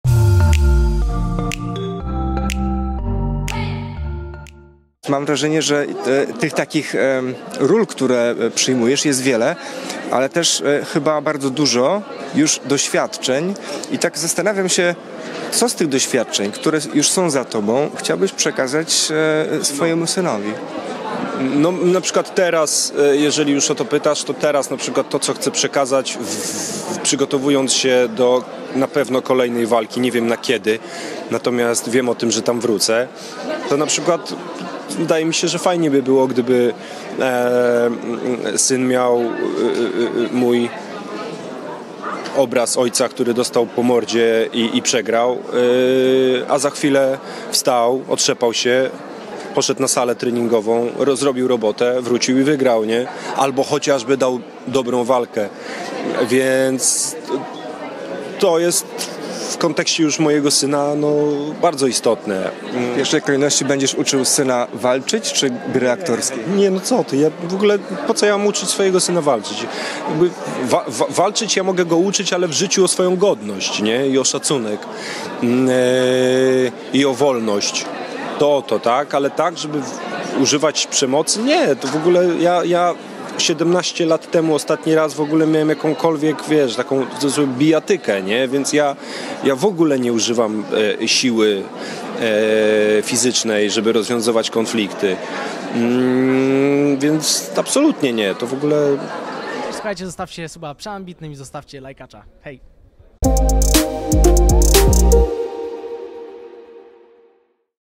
Sebastian Fabijański TEGO nauczy SYNKA - MAFF będzie zadowolona? | przeAmbitni.pl
Wywiad z Sebastianem Fabijańskim, Warszawa Premiera filmu Heaven in Hell-   08.02.23 r.